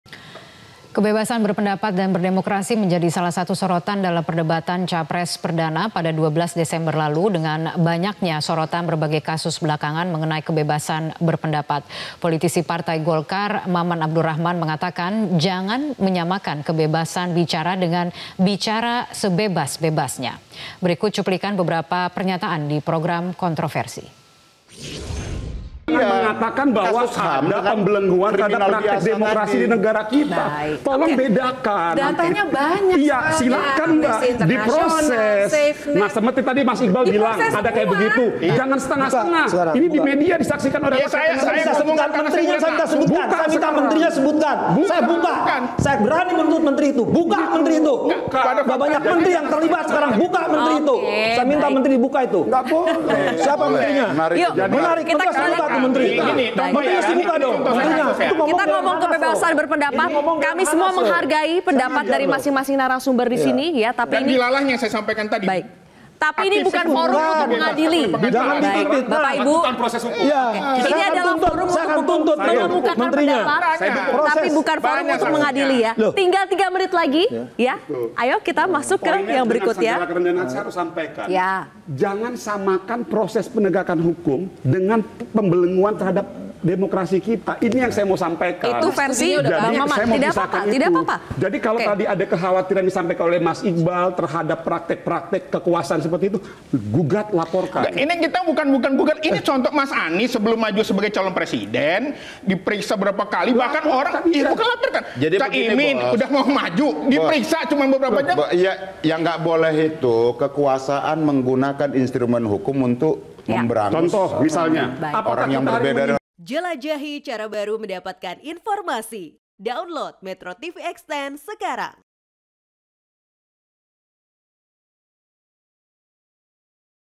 Soal Kebebasan Berpendapat Emosi Politisi Golkar Memanas
MetroTV, Kebebasan berpendapat dan berdemokrasi menjadi salah satu sorotan dalam perdebatan capres perdana pada 12 Desember lalu dengan banyaknya sorotan berbagai kasus belakangan mengenai kebebasan berpendapat. Politisi Partai Golkar Maman Abdurrahman mengatakan jangan menyamakan kebebasan bicara dengan bicara sebebas-bebasnya.